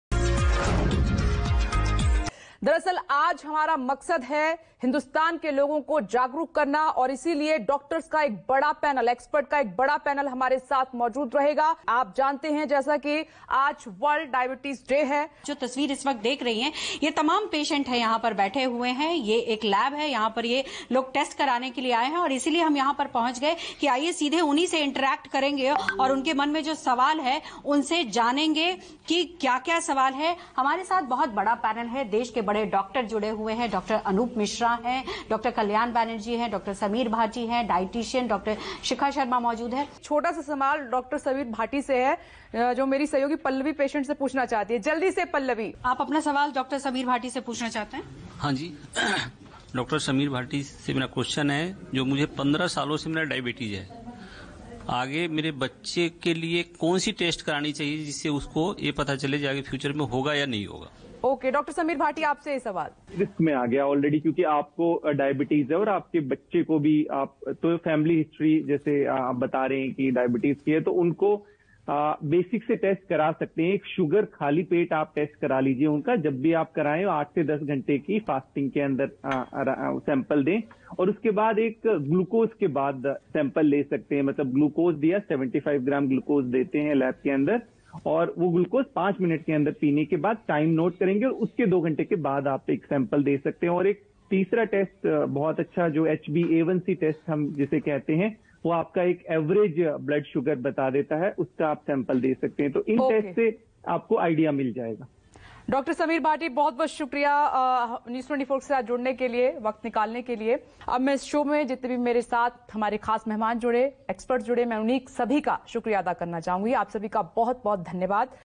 Is Diabetes Genetic | Dr Sameer Bhati
Access To Diabetes Education and Care throws the light on awareness about Diabetes.  Nowadays the effects of Diabetes has been seen in Teenagers Also.

Let's Know how this disease can be diagnosed early and prevented.

A brief Discussion with News24.